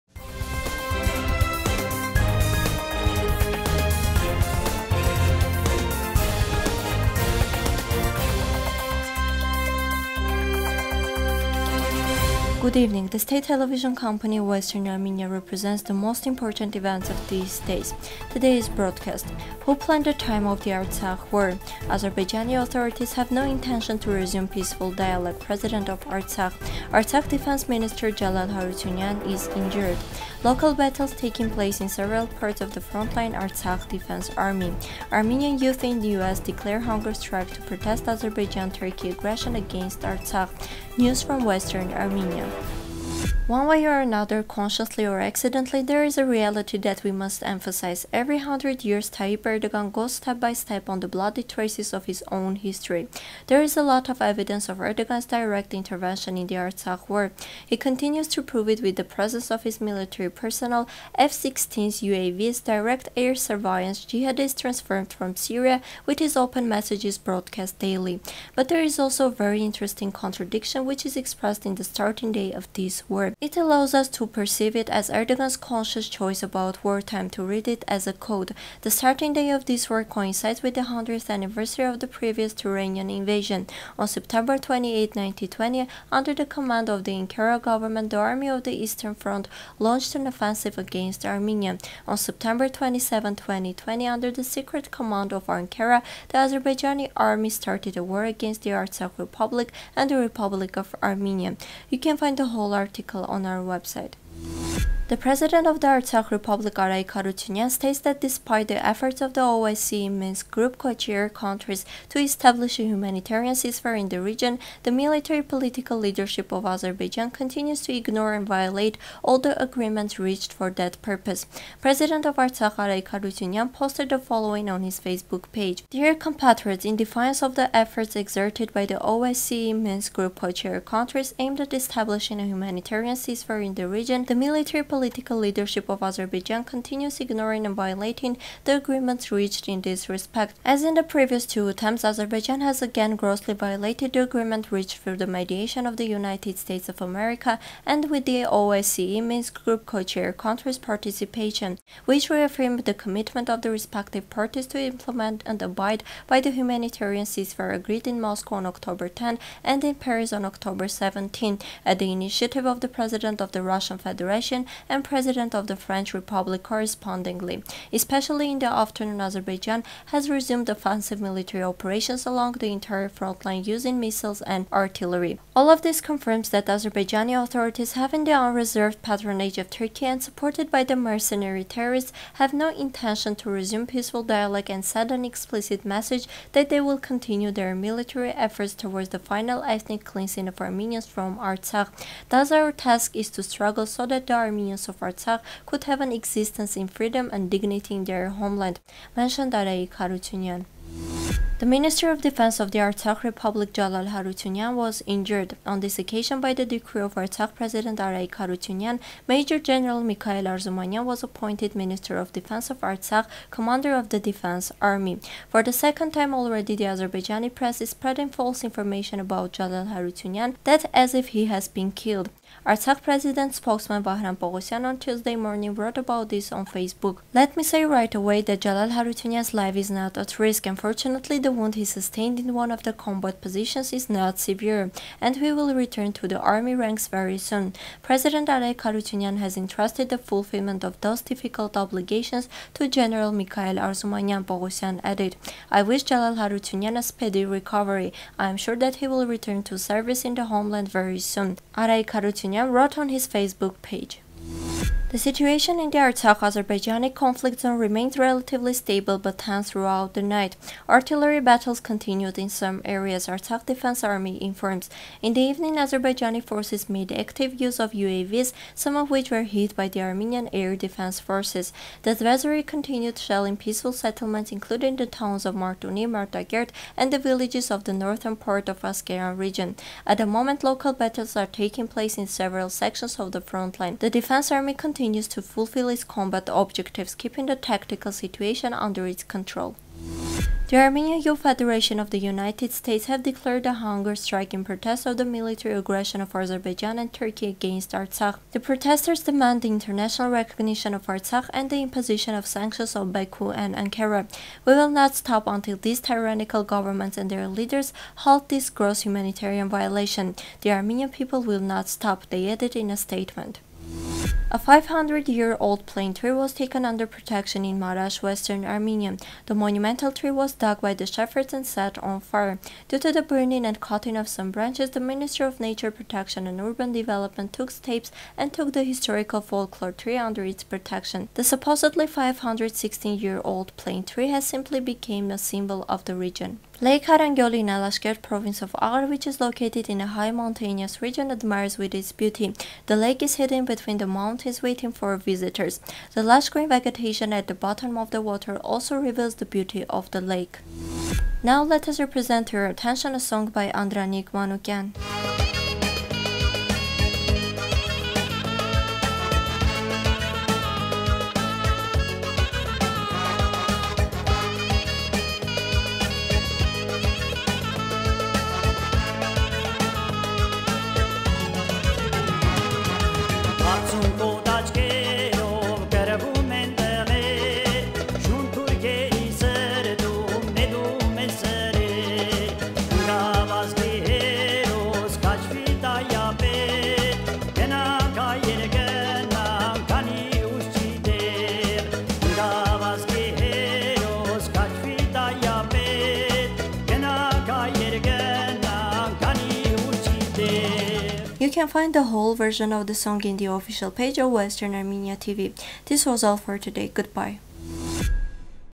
Armenian youth in US declared hunger strike to protest Azerbaijan․NEWS 27.10.2020
1.Who planned the time of the Artsakh war?
2.Artsakh President: Azerbaijani authorities have no intention to resume peaceful dialogue
3.Artsakh Defense Minister Jalal Harutyunyan is injured
4․Local battles taking place in several parts of the front line – Artsakh Defense Army
5.Armenian youth in US declared hunger strike to protest Azerbaijan, Turkey aggression against Artsakh
6.News from Western Armenia